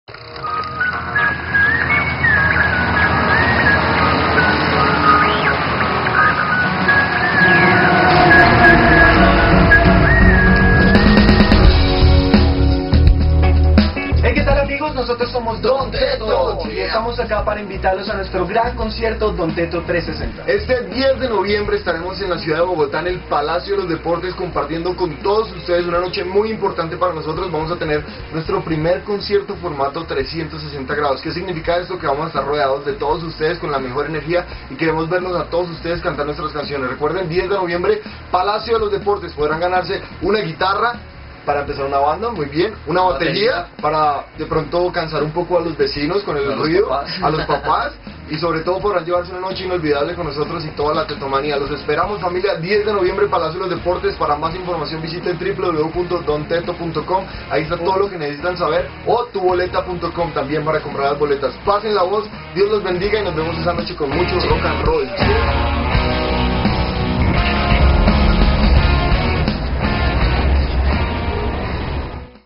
DON TETTO
Palacio de Los Deportes

DON TETTO UNA ENERGIZANTE PROPUESTA DESDE COLOMBIA
Nacida en Marzo del 2003 en Bogotá Colombia, Don Tetto es una de las bandas más populares del país cafetero, con una convocatoria que los ha llevado a realizar decenas de presentaciones y a ganar un premio MTV y compartir el escenario con artistas internacionales como Green Day, Fall Out Boy y los más importantes de Colombia, entre muchos otros logros.